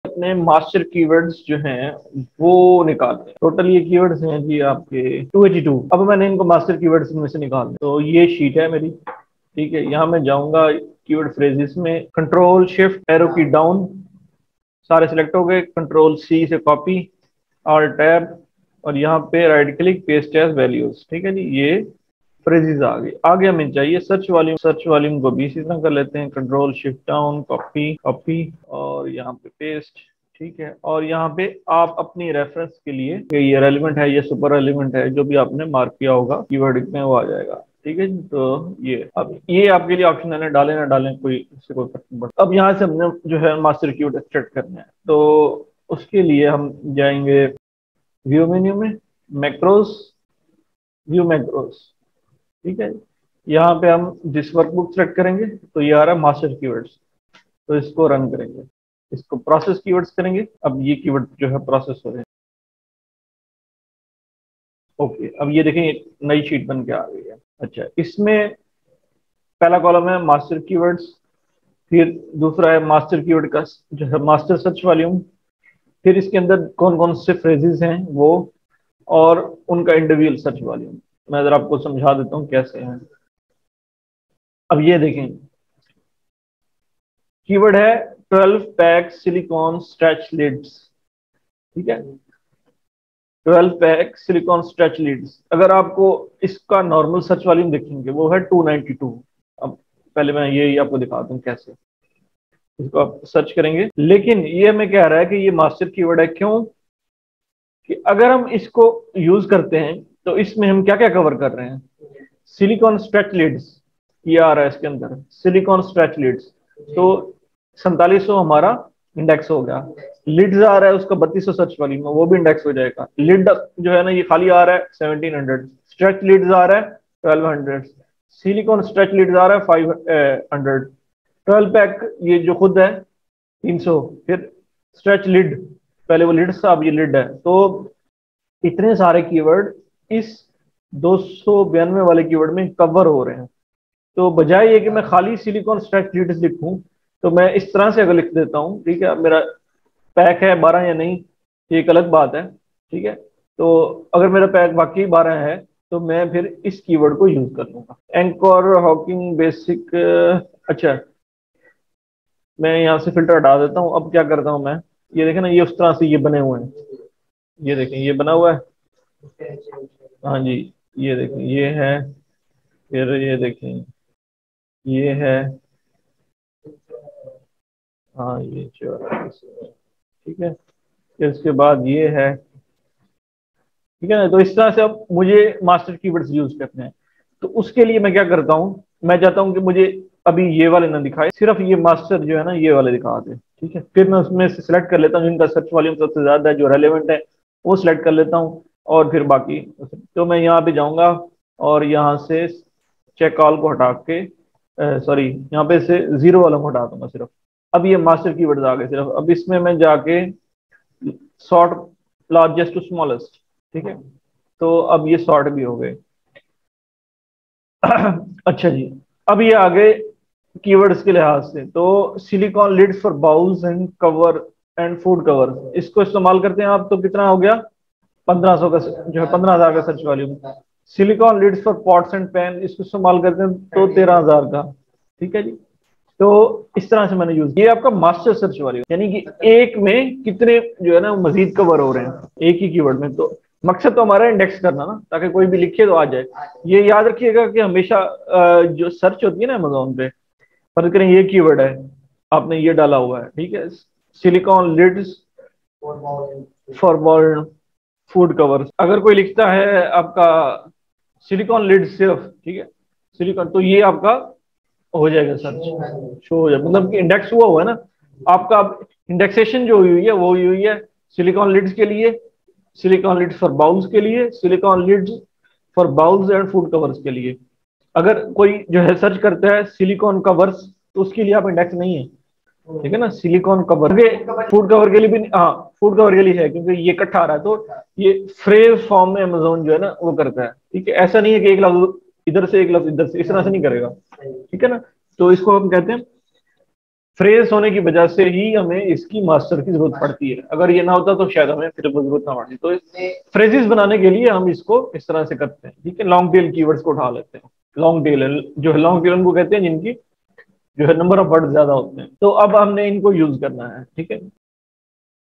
Cracking the Code: Unveiling the Power of Master Keywords for Amazon SEO
Complete Course on 'Alibaba Product Sourcing For Amazon FBA in 2022'

Dive into the world of master keywords! Understand their significance in optimizing your Amazon listings for search. Explore practical examples and strategies in Urdu/Hindi to boost your product's discoverability.